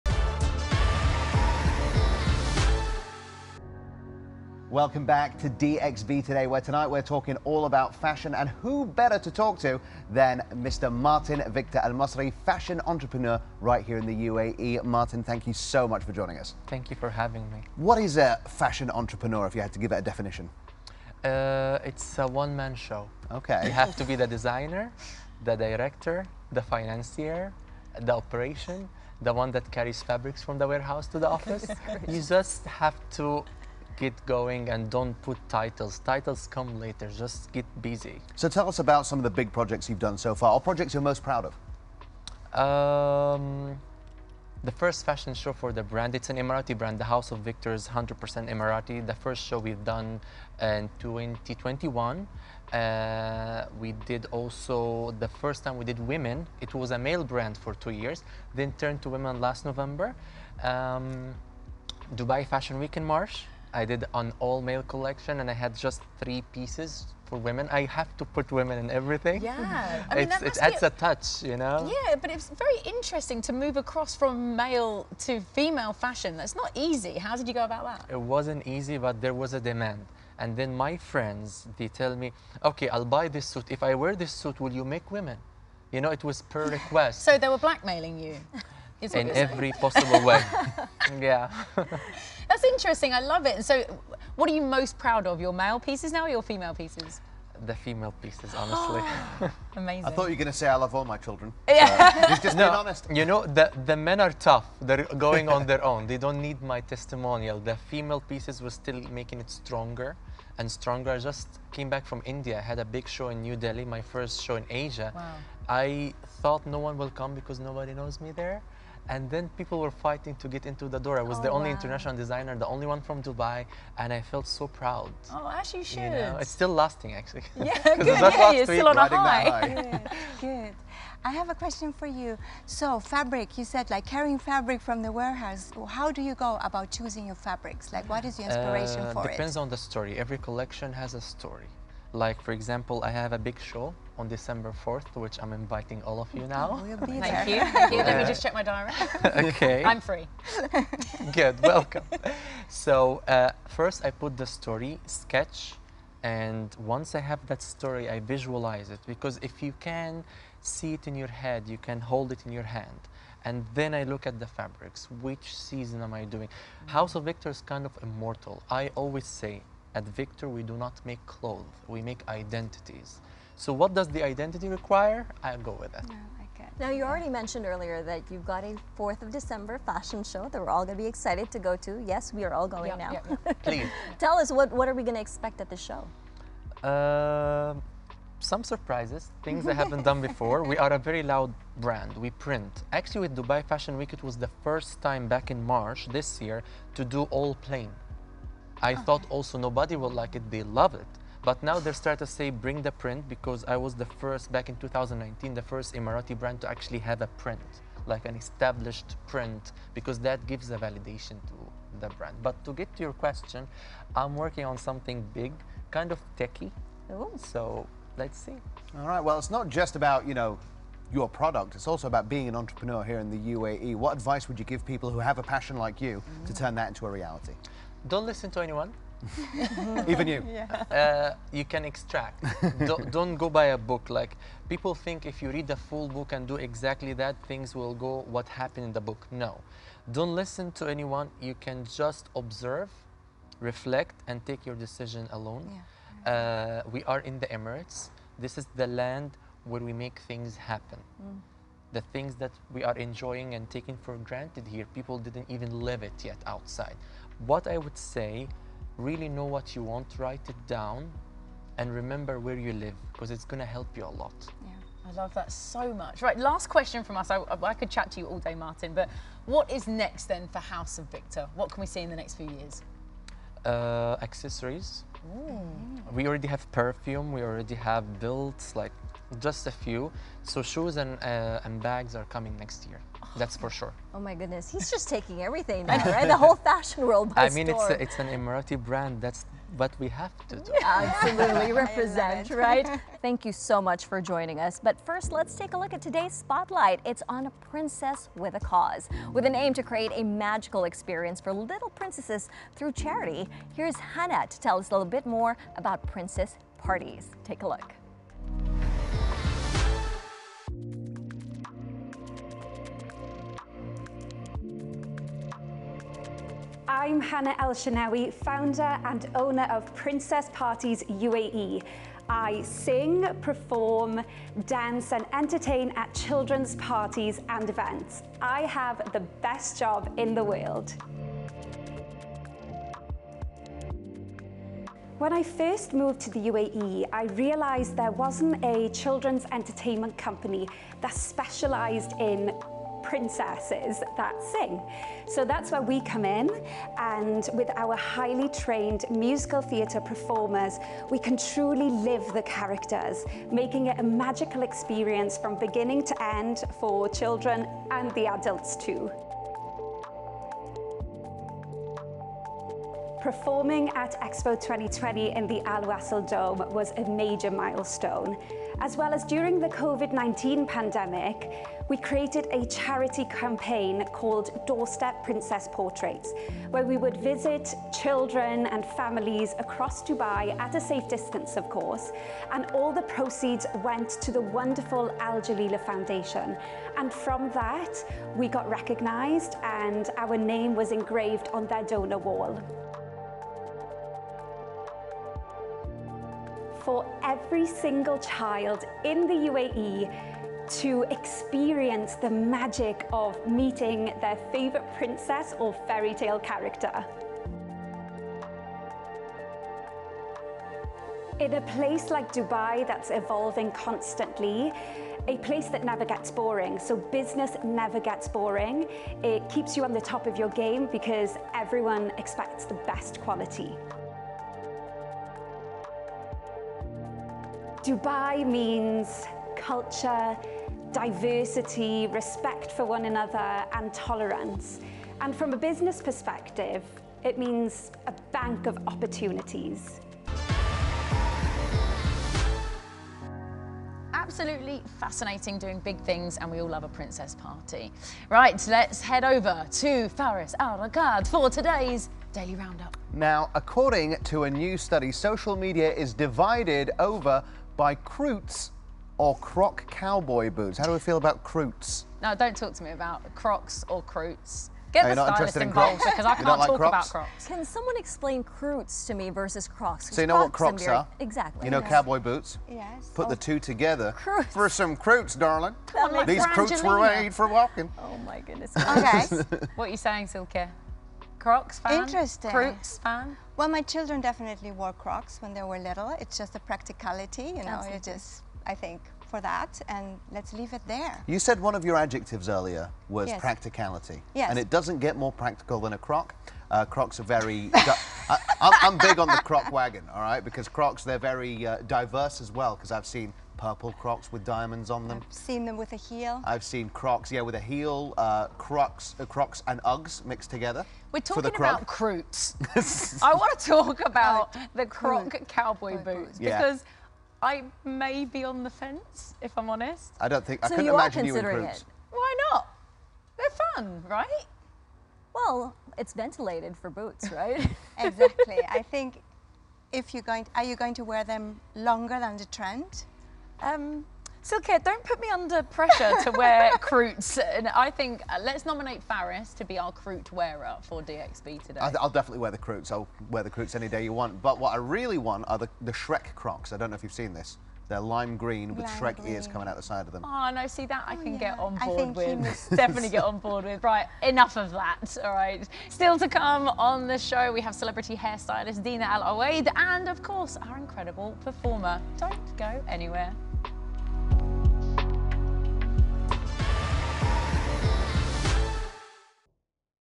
A fashion mogul's playbook: How to make your brand soar
Fashion designer Martin Victor shares his secrets to creating a successful clothing brand